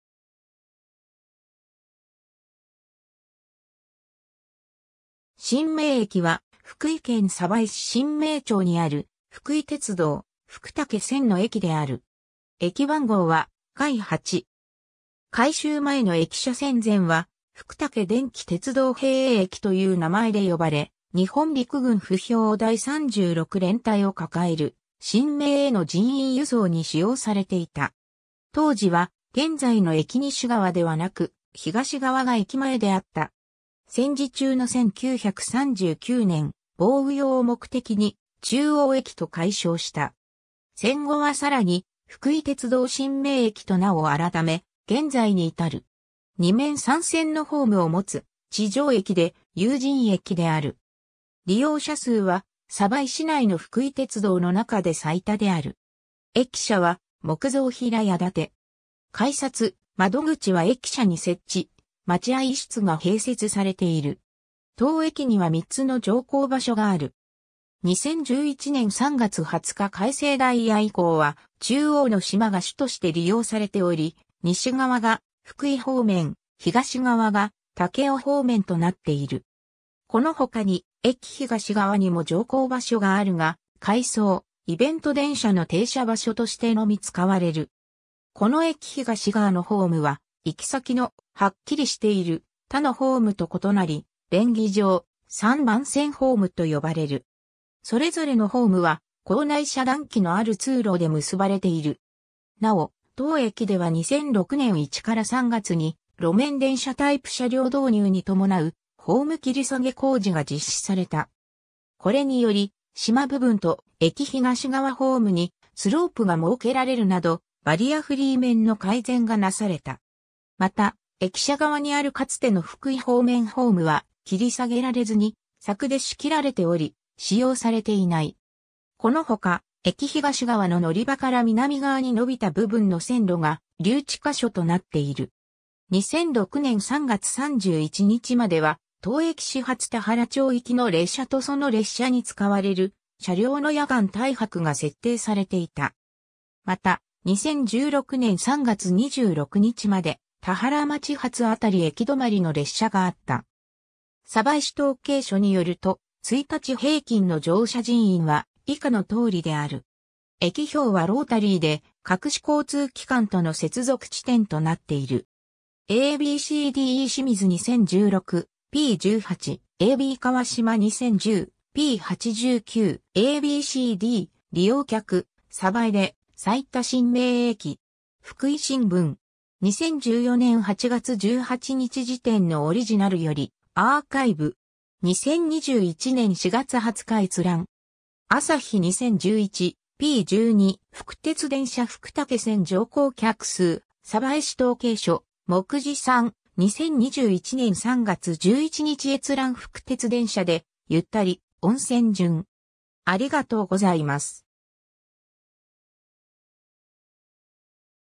神明駅 (福井県)
神明駅（しんめいえき）は、福井県鯖江市神明町にある、福井鉄道福武線の駅である。
駅番号はF8。
 改修前の駅舎（2009年6月6日） 戦前は、福武電気鉄道兵営駅という名前で呼ばれ、日本陸軍歩兵第36連隊を抱える神明への人員輸送に使用されていた。
当時は現在の駅西側ではなく、東側が駅前であった。
 戦時中の1939年、防諜を目的に「中央駅」と改称した。
戦後はさらに福井鉄道神明駅と名を改め、現在に至る。
 2面3線のホームを持つ地上駅で、有人駅である。
利用者数は鯖江市内の福井鉄道のなかで最多である。
駅舎は木造平屋建て。
改札・窓口は駅舎に設置、待合室が併設されている。
 当駅には3つの乗降場所がある。
2011年3月20日改正ダイヤ以降は中央の島が主として利用されており、西側が福井方面、東側が武生方面となっている。
このほかに、駅東側（旧駅舎跡）にも乗降場所があるが、回送・イベント電車の停車場所としてのみ使われる。
この駅東側のホームは行先のはっきりしている他のホームと異なり、便宜上、3番線ホームと呼ばれる。
 それぞれのホームは構内遮断機のある通路で結ばれている。
 なお、当駅では2006年1 - 3月に路面電車タイプ車両導入に伴うホーム切り下げ工事が実施された。
これにより、島部分と駅東側ホームにスロープが設けられるなど、バリアフリー面の改善がなされた。
また、駅舎側にあるかつての福井方面ホームは切り下げられずに、柵で仕切られており、使用されていない。
 このほか、駅東側の乗り場から南側に延びた部分の線路が留置箇所となっている。
 2006年3月31日までは当駅始発田原町行きの列車とその列車に使われる車両の夜間滞泊が設定されていた。
また、2016年3月26日まで田原町発当駅止まりの列車があった。
 「鯖江市統計書」によると、1日平均の乗車人員は以下の通りである。
 駅表（西側）はロータリーで、各種交通機関との接続地点となっている。
 ^ a b c d e 清水 2016, p. 18. ^ a b 川島 2010, p. 89. ^ a b c d “利用客、鯖江で最多 神明駅（鯖江市神明町2丁目）”. 福井新聞 (2008年7月5日). 2014年8月18日時点のオリジナルよりアーカイブ。
2021年4月20日閲覧。
 ^ 朝日 2011, p. 12. ^ 福鉄電車福武線乗降客数 鯖江市統計書　目次3、2021年3月11日閲覧 ^ 福鉄電車で湯ったり 温泉巡